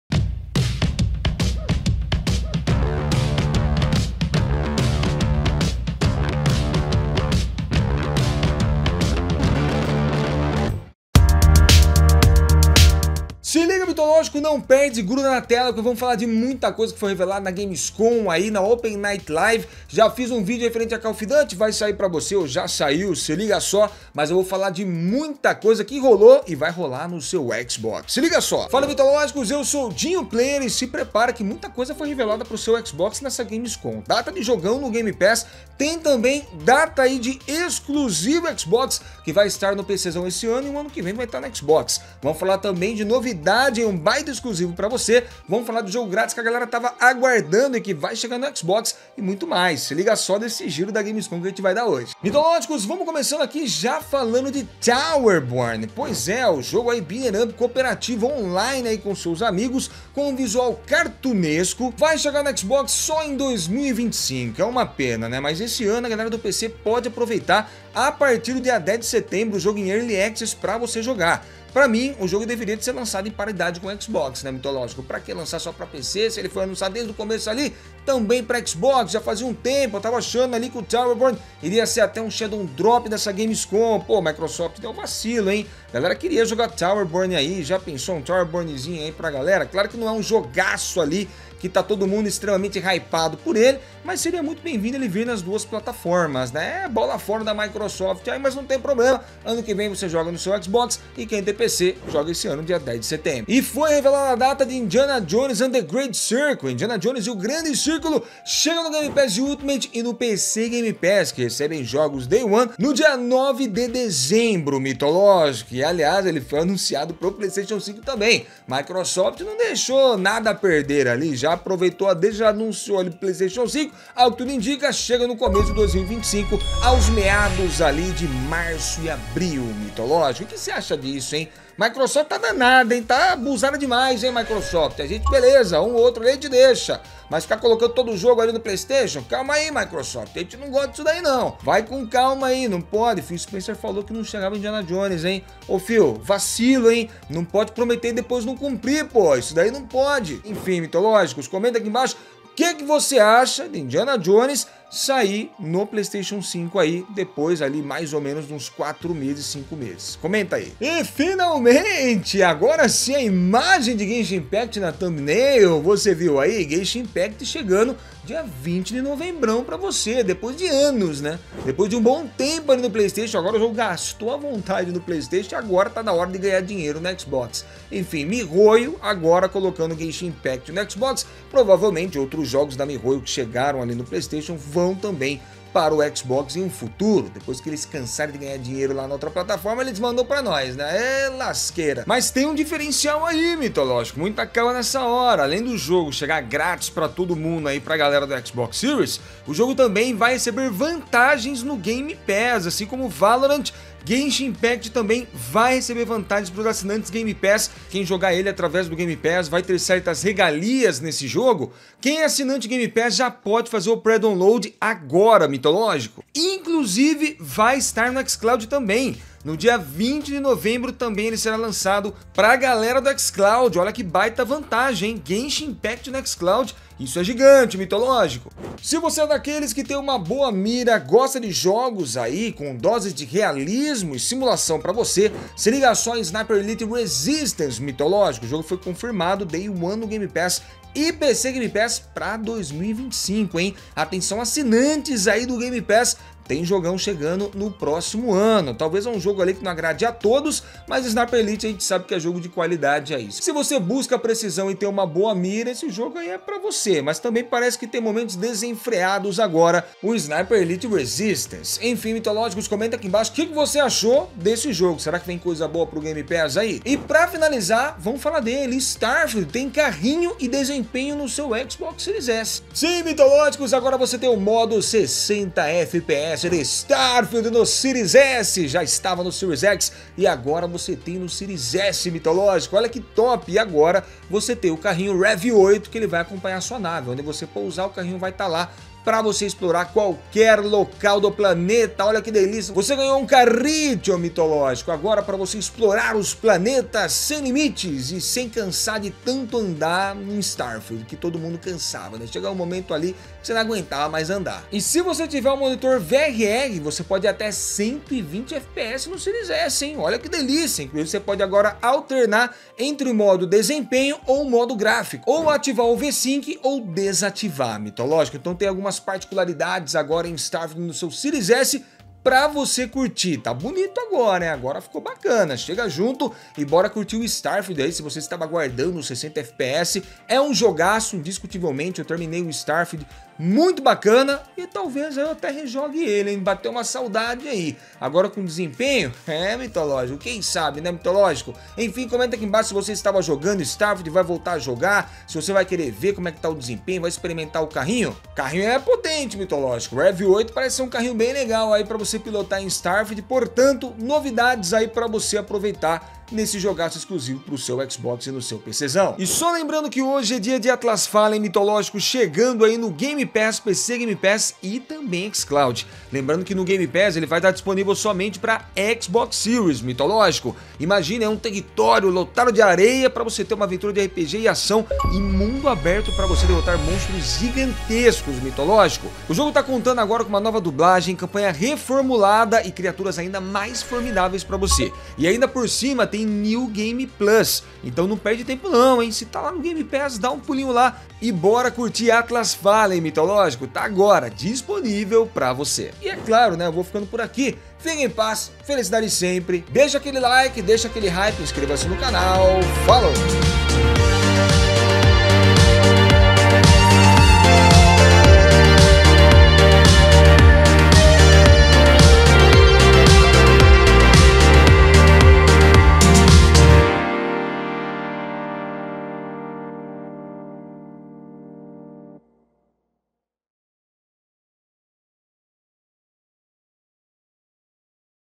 GENSHIN no XBOX e XCLOUD | ANÚNCIOS de JOGOS para o GAME PASS, PC GAME PASS | TEM JOGÃO HOJE!
Se liga Mitológico por que tem Genshin Impact no seu Xbox e XCloud, anúncios de jogos para o Game Pass, PC Game Pass e muitas novidades para você conferir e ainda tem jogão liberado hoje pra você baixar e jogar. Confira no Mais Xbox!

Não se esqueça de deixar o seu LIKE e caso ainda não seja um inscrito aqui do canal, BORA SE INSCREVERRRRR!

Vem jogar com a gente:
Gamertag: DinhoPlayer